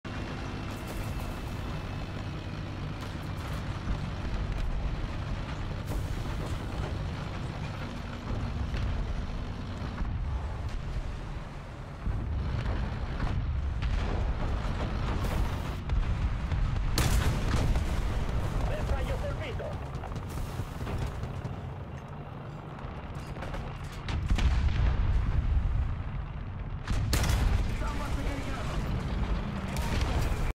Bad WiFi + Teammates = Worst Enemies
Don't you just love it when your WiFi hates the idea of playing a game?

(The snail has doomed my router)